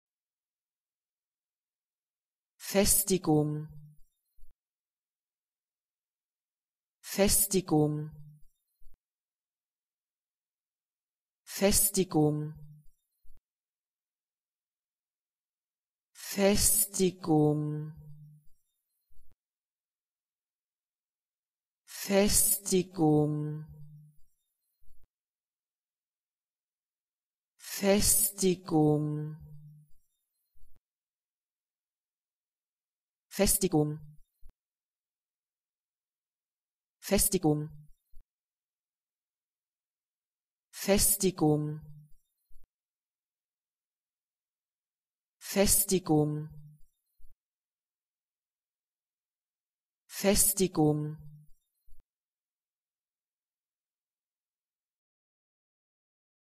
How to pronounce Festigung in German - Perfectly
This video shows you how to pronounce Festigung in German , pronounce and pronunciation how-to guide.
Watch and learn  how to say this word rightly with my channel, how to pronounce Festigung  free pronunciation videos.

What does this Festigung mean ? Words’ meaning, dictionary description, information, explanation.
consolidation, strengthening, establishment, reinforcement, consolidating, firming, solidifying, cementing, strengthened, consolidate, strengthen, reinforce, reinforcing, stabilisation, fortification

1 .EN The consolidation(Festigung) of our sales will be the focus again next year.
1 .DE Die Festigung(consolidation) unseres Vertriebes steht nächstes Jahr wieder im Fokus.

2 .EN Therefore, it is easier for us to remember what it means to achieve two aspirations: membership of the European Union, and the consolidation(Festigung) of a system of democratic liberties.
2 .DE Uns ist also die Erfüllung zweierlei Hoffnung noch sehr frisch im Gedächtnis: Beitritt zur Europäischen Union und Festigung(consolidation) eines Systems demokratischer Freiheiten.

3 .EN In the body it promotes the strengthening(Festigung) of dental enamel.
3 .DE Es begünstigt im Körper die Festigung(strengthening) des Zahnschmelzes.

00:00 - Normal Festigung  Pronounce
00:10 - How to pronounce Festigung ?